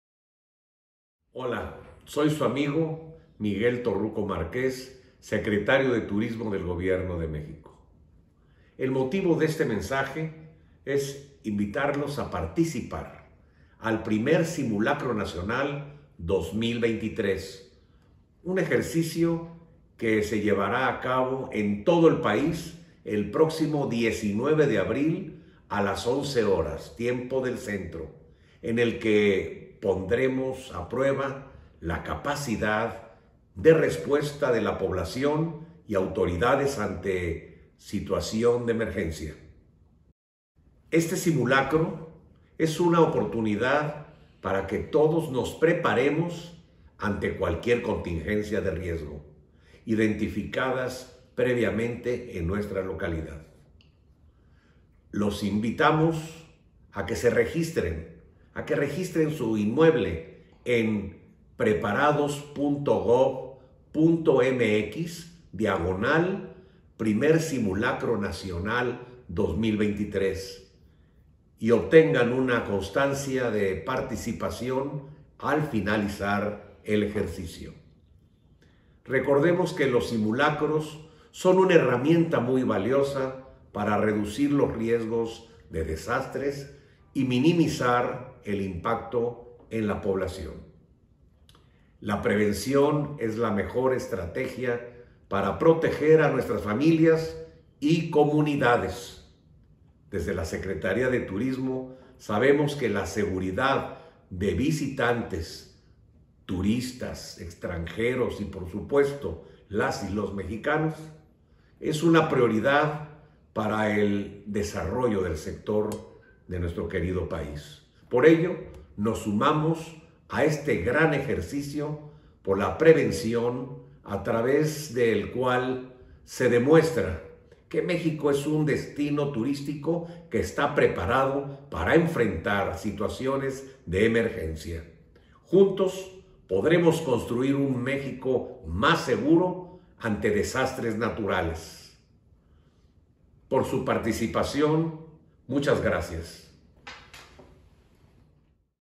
1er Simulacro Nacional 2023-
El secretario de Turismo, Miguel Torruco Marqués invita a todos a participar en este primer simulacro a realizarse el  próximo 19 de abril a las 11:00 horas tiempo del centro de México.